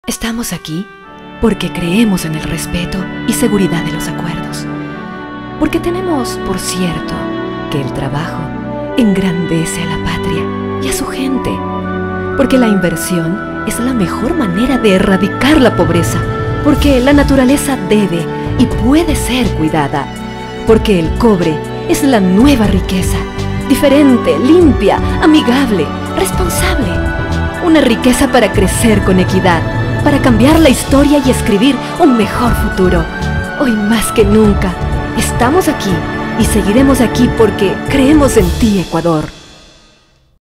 ESTAMOS AQUI FINAL corregido DV NTSC
Spot ECSA Ecuacorriente S.A. Escrito y dirigido por Francisco Cevallos Lupera. Dirección de Fotografía Francisco Salazar, Producción Liza Chitiva